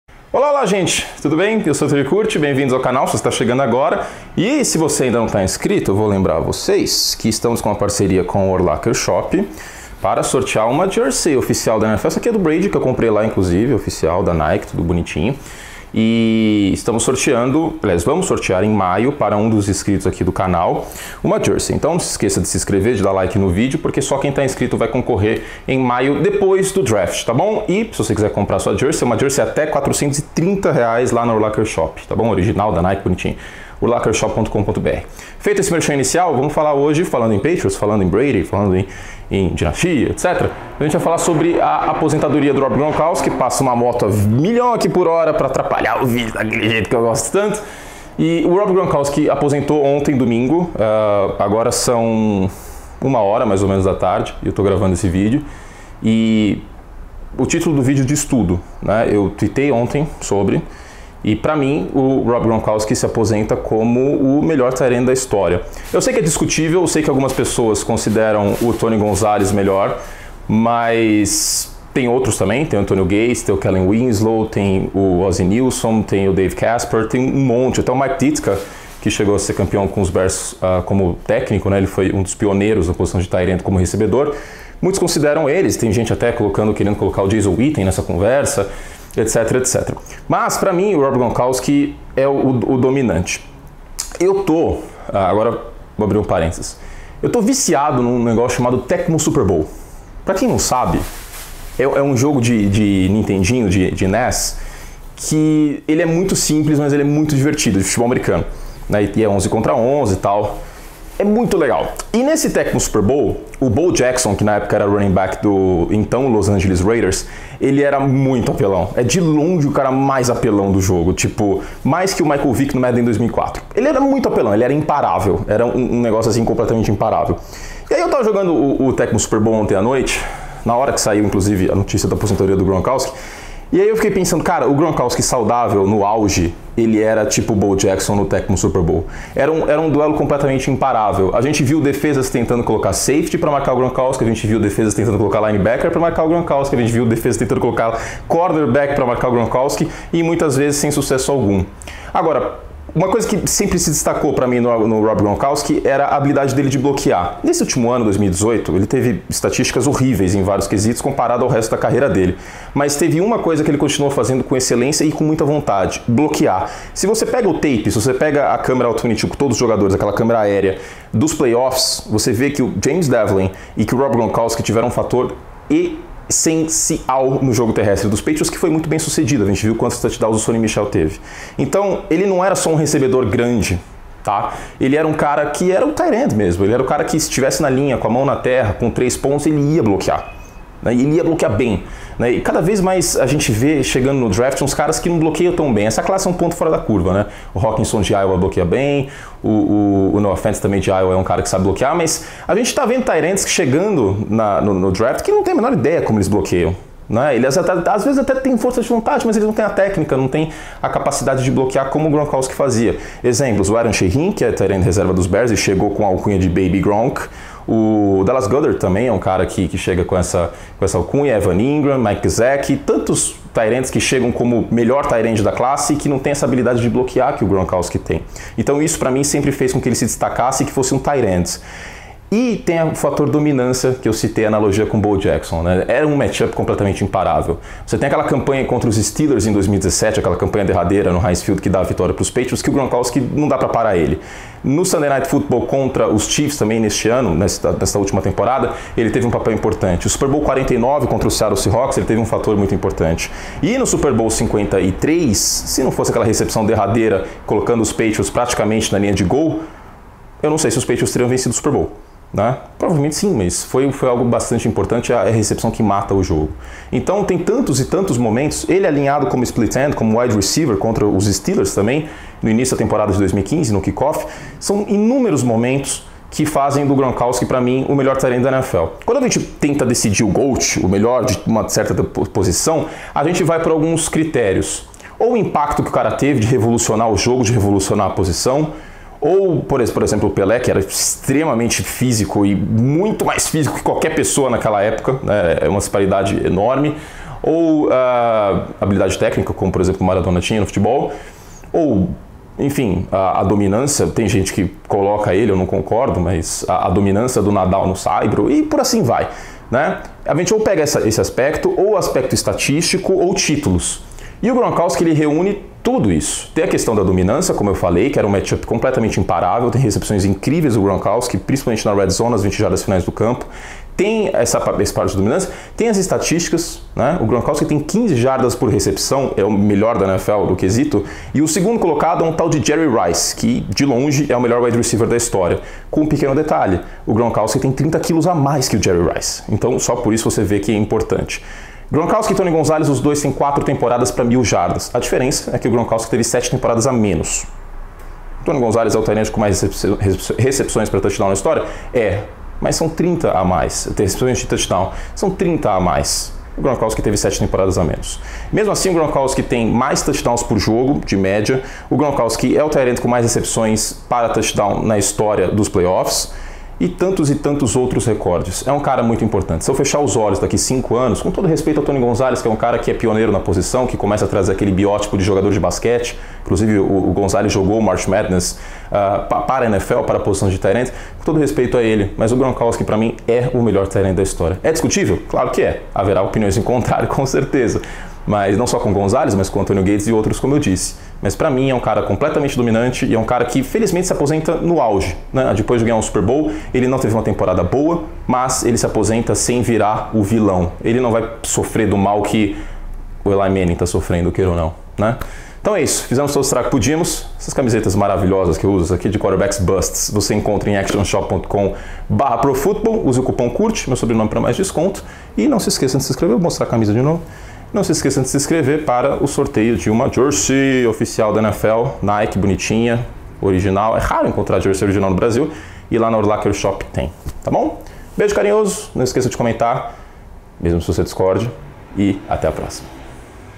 Rob Gronkowski aposenta como melhor tight end da história
Na noite de domingo as especulações se confirmaram e Rob Gronkowski decretou aposentadoria. Gronkowski aposenta como o melhor da história? É o que falo neste vídeo.